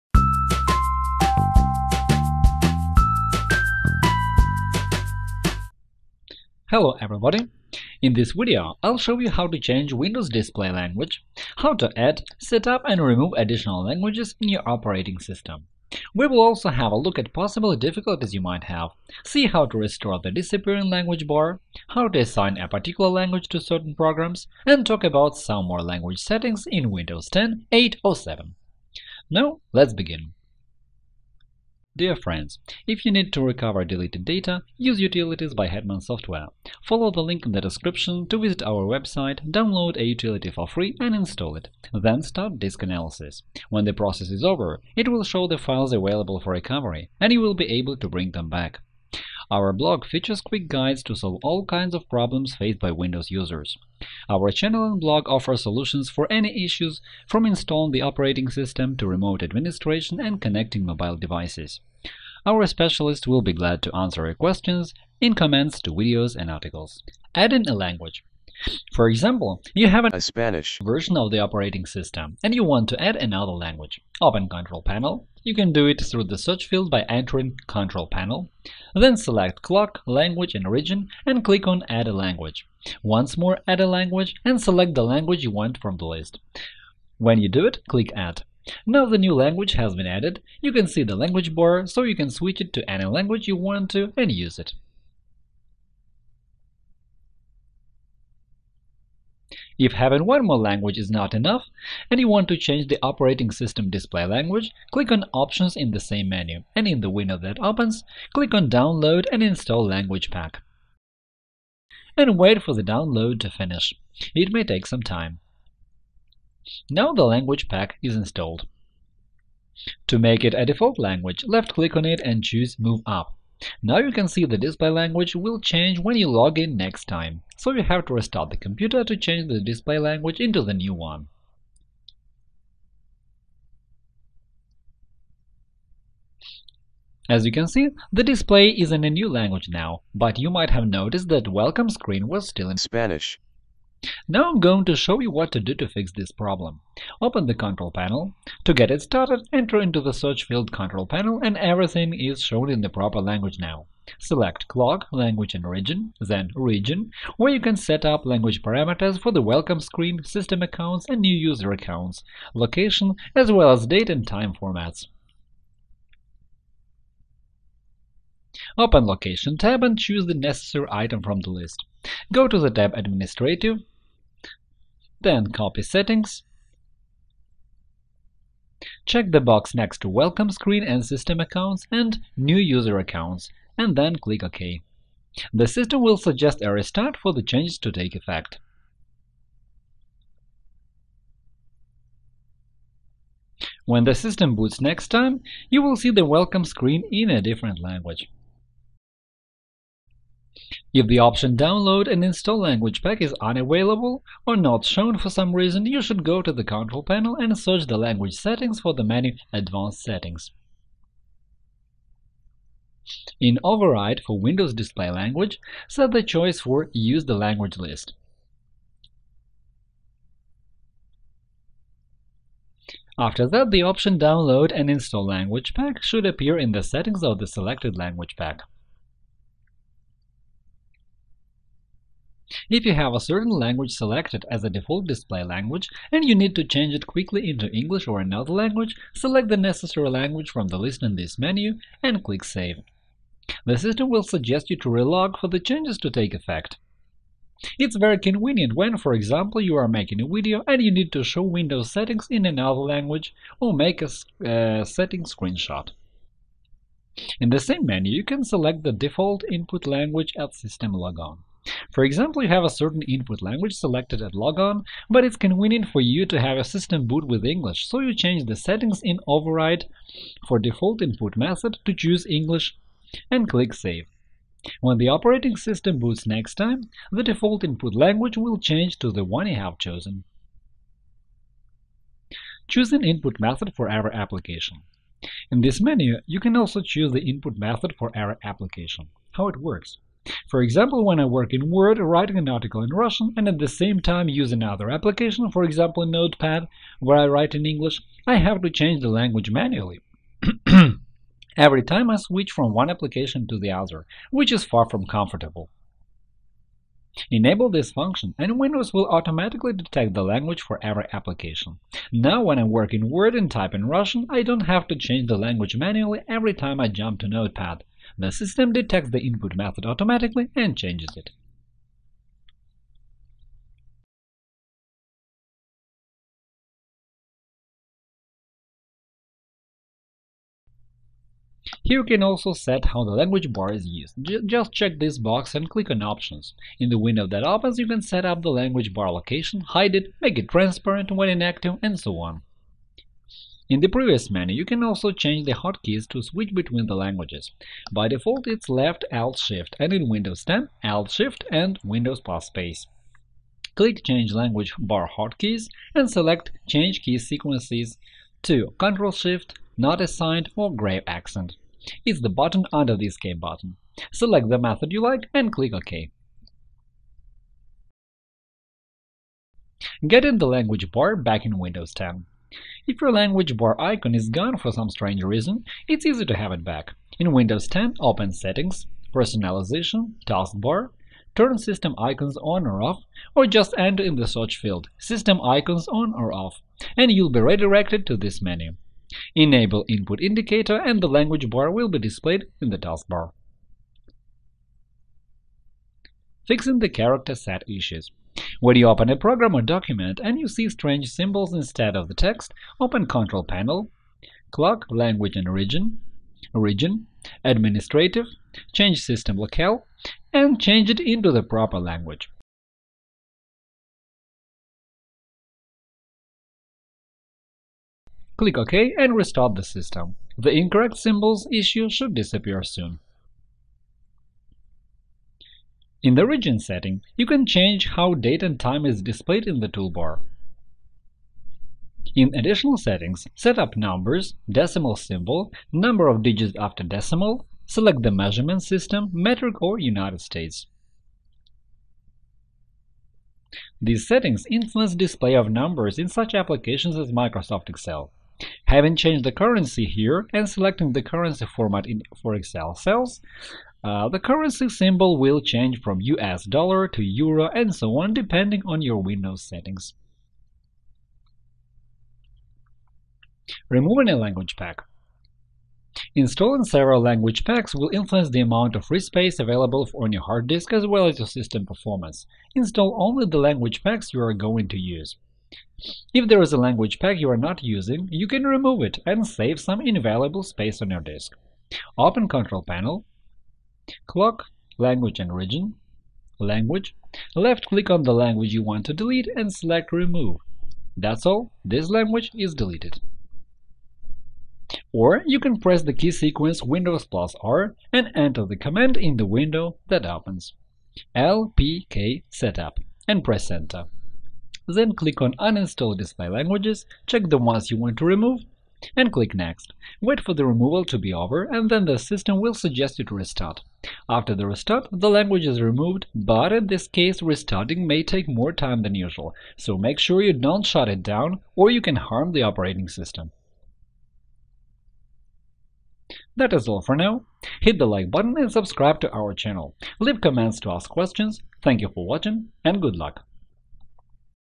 How to Add a Language, Change Shortcuts or Display Language in Windows 10, 8 or 7 🇺🇸 🇪🇸 🇺🇦
👍 Watch how to change Windows display language, how to add, set up and remove additional languages in your operating system.  We will also have a look at possible difficulties you might have, see how to restore the disappearing language bar, how to assign a particular language to certain programs, and talk about some more language settings in Windows 10, 8 or 7.

Contents:
0:00 - Intro;
1:08 - Adding a language;
1:39 - Changing the display language;
3:20 - Troubleshooting when downloading the language pack and changing the display language;
4:17 - Choosing default input method;
4:45 - Choosing input method for every application;
5:31 - Using key shortcuts to switch between input languages;
6:12 - Getting the language bar back in Windows 10;
6:38 - Fixing the character set issues;
7:05 - Region;
7:45 - Removing a language pack;

That is all for now.  Leave comments to ask questions. Good luck.